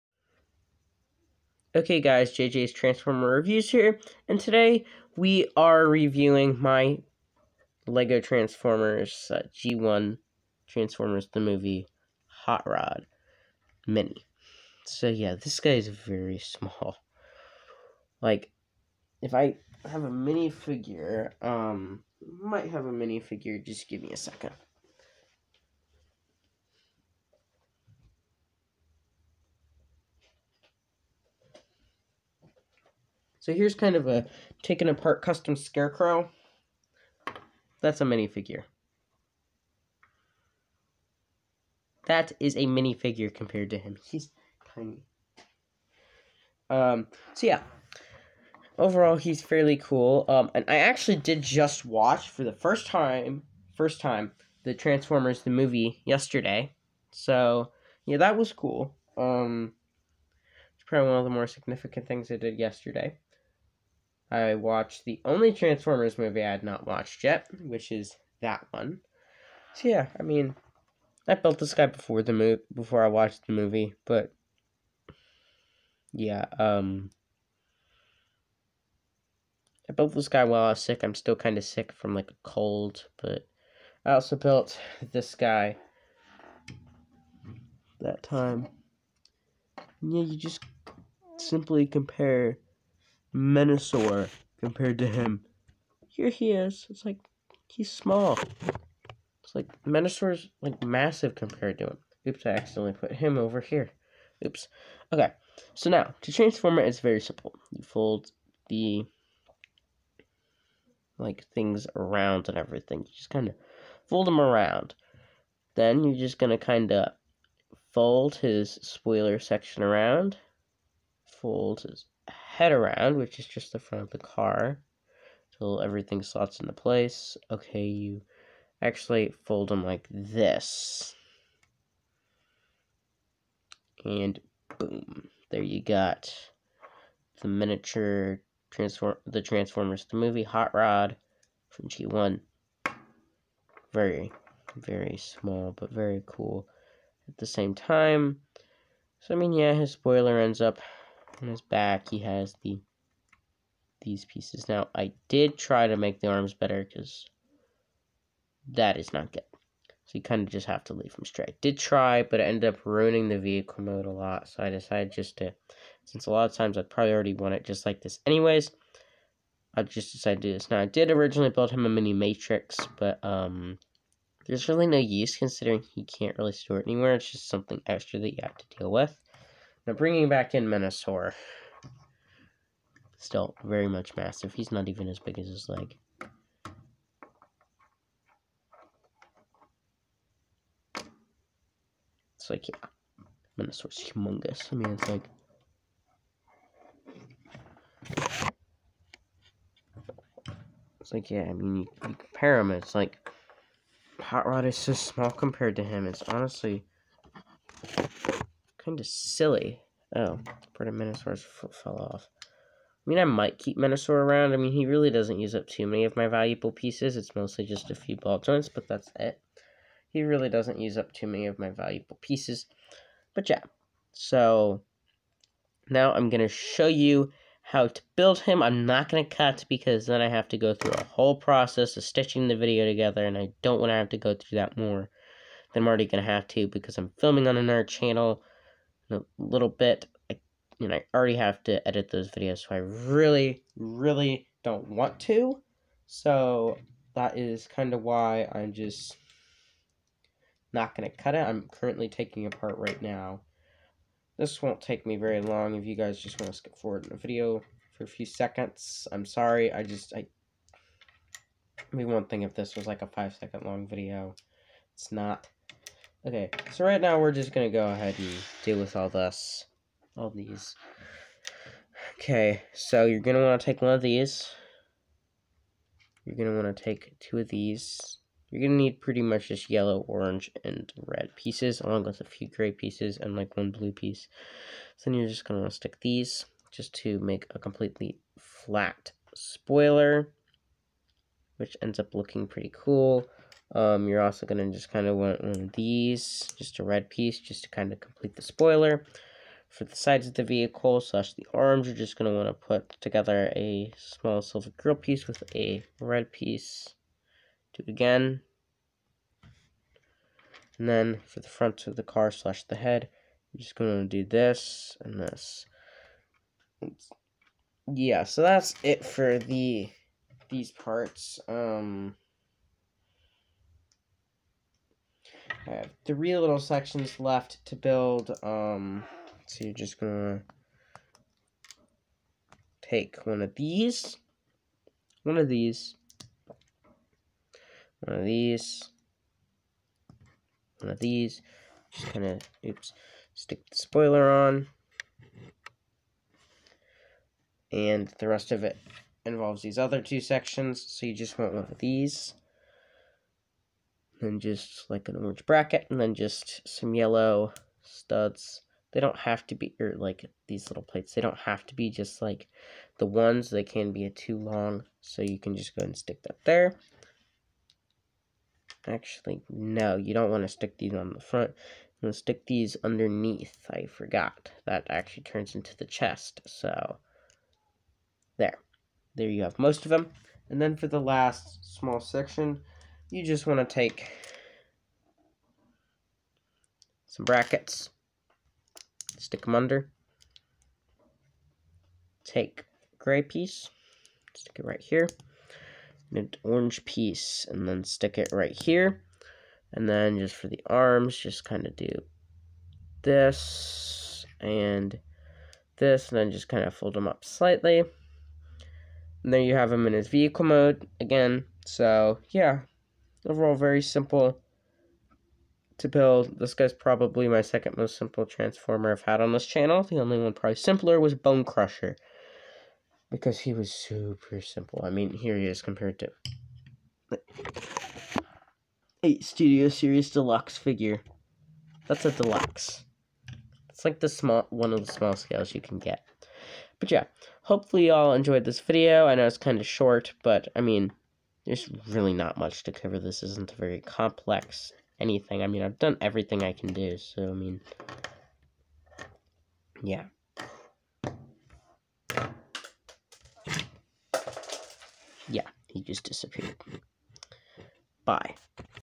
lego transformers g1 hot rod plus tutorial
um hot rod was pretty much fine after i flicked him off my desk besides one of the silver parts was missing so i just got a new one